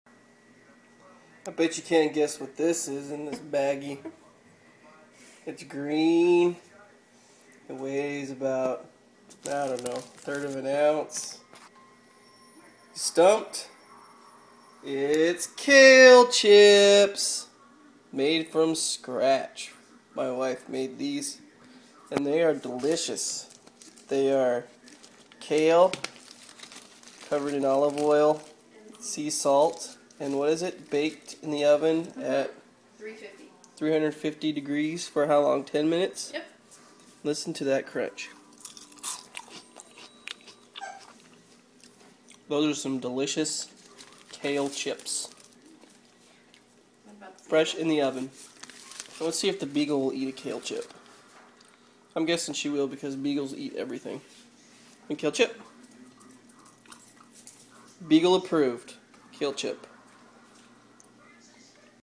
These are Kale Chips Sucka!
This is how you can make your very own Kale Chips in the oven.  These tasty Kale Chips are good for a healthy snack anytime.  Here is the recipe for nutritious Kale Chips.

Kale
Olive Oil
Sea Salt

Cut up Kale to bite sized pieces, blend with a tablespoon olive oil and tablespoon sea salt.  Bake on a cookie sheet for 10 minutes at 350 degrees.  Let cool and then enjoy.  This is a nice vegetarian snack and way better for you then potato chips.